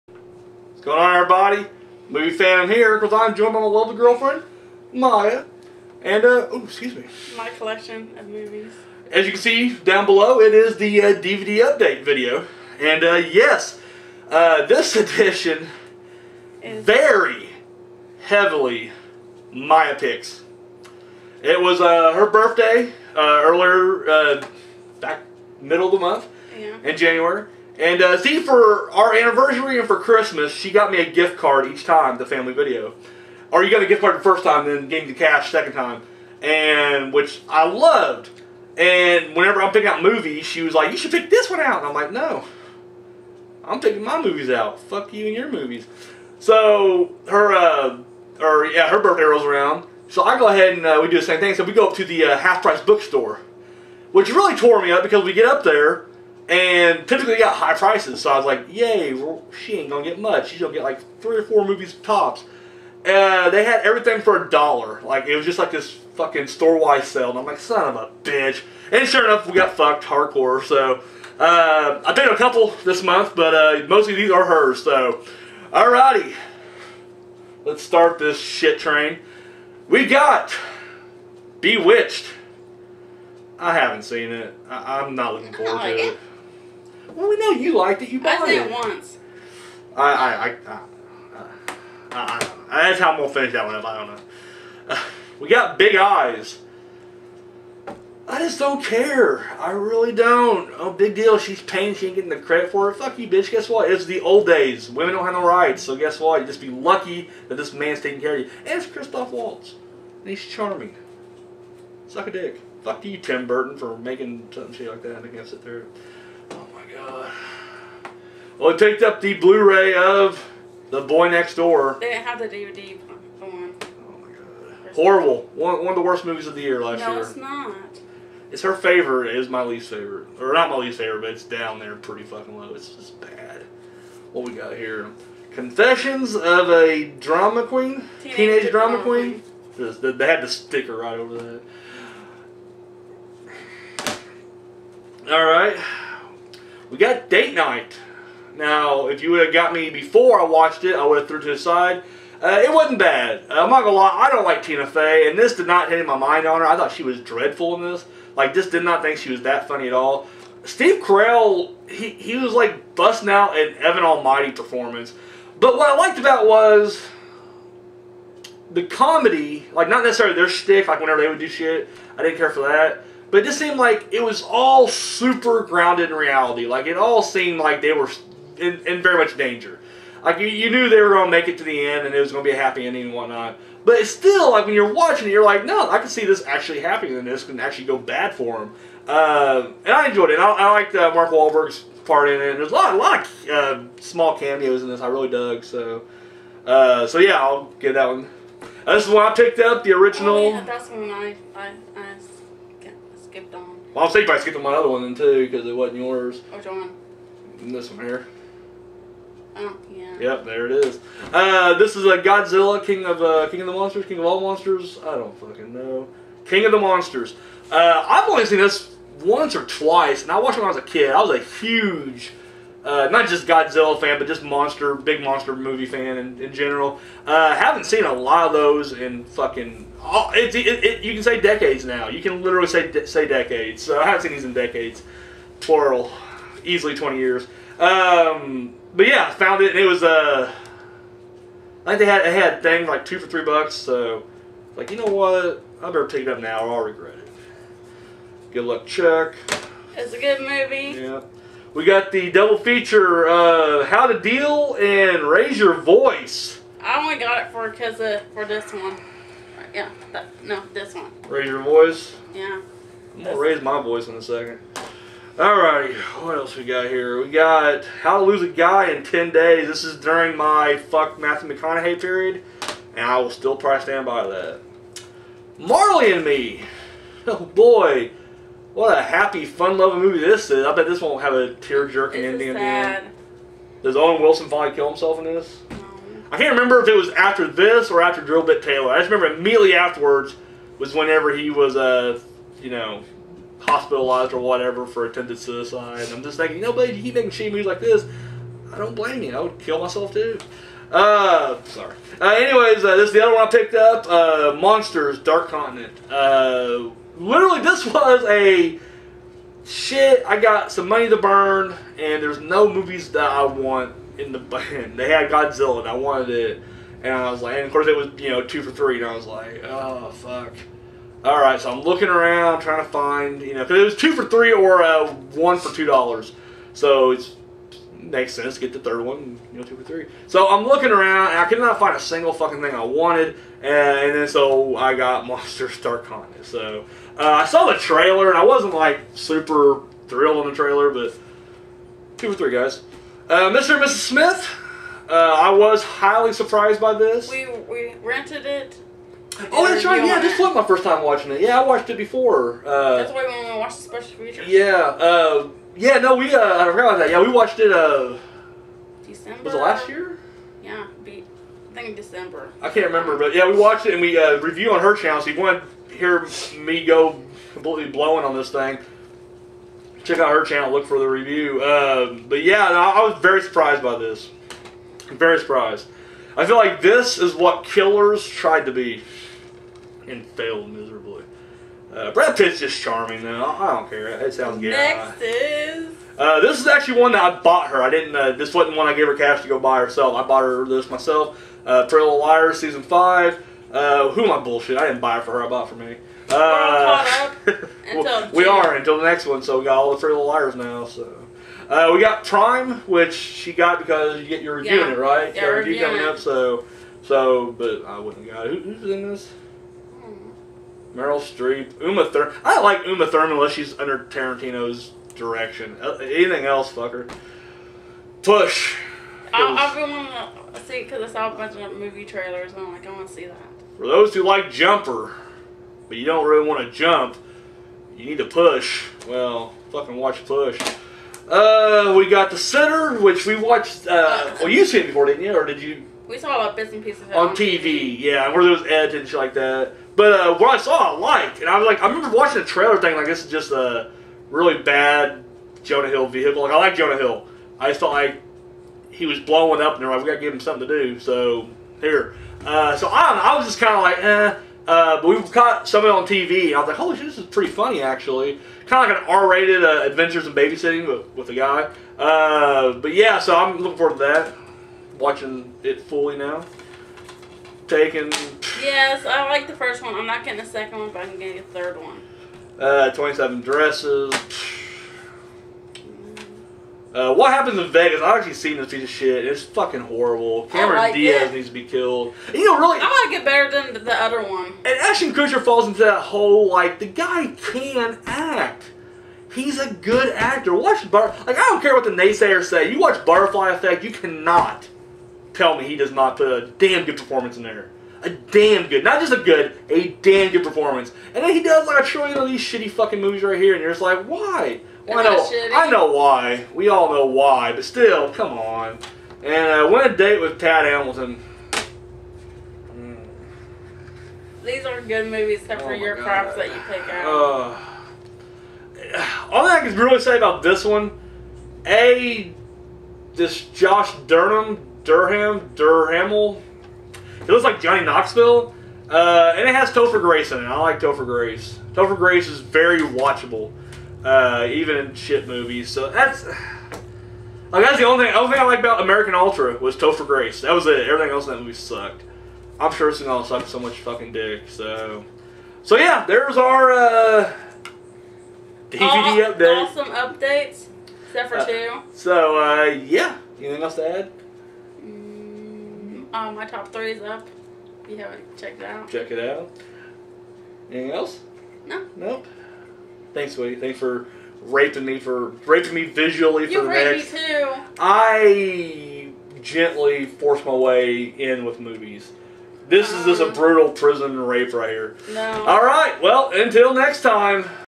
DVD Update 2/16
A special Mya edition of a DVD Update!  Don't worry, I still run my mouth and trash her films!